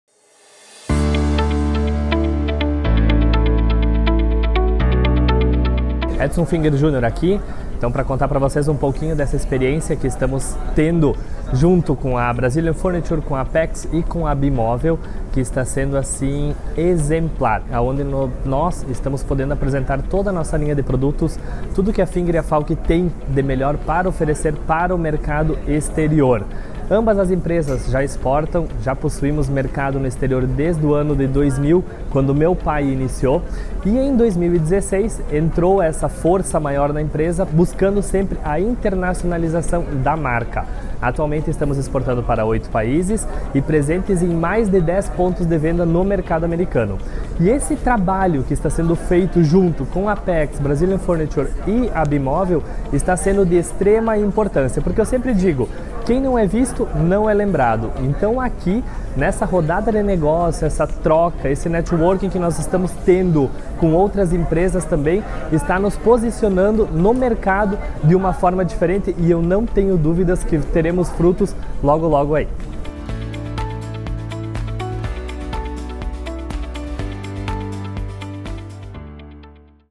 Edson Finger Junior - Falkk | Projeto Comprador Movelpar | Brazilian Furniture
Projeto Comprador Movelpar | Brazilian Furniture
A Movelpar Home Show 2024 aconteceu no polo moveleiro de Arapongas (PR), reunindo 160 expositores. Com o objetivo de atrair compradores qualificados de dentro e fora do Brasil, entre as principais ações do escopo da feira esteve o Projeto Comprador, organizado pela ABIMÓVEL em parceria com a ApexBrasil por meio do Projeto Brazilian Furniture.

Neste ano o Projeto Comprador conectou 54 marcas associadas a 15 compradores internacionais provindos de dez diferentes países: Bolívia, Chile, Colômbia, Estados Unidos, Marrocos, Panamá, Paraguai, Reino Unido, República Dominicana e Uruguai.

As rodadas de negócios do Projeto Comprador Movelpar Home Show 2024 ocorreram durante o dia 30 e 31 de janeiro, no Expoara, centro de eventos que recebe a feira.

MÓVEIS: O NOSSO NEGÓCIO! 🇧🇷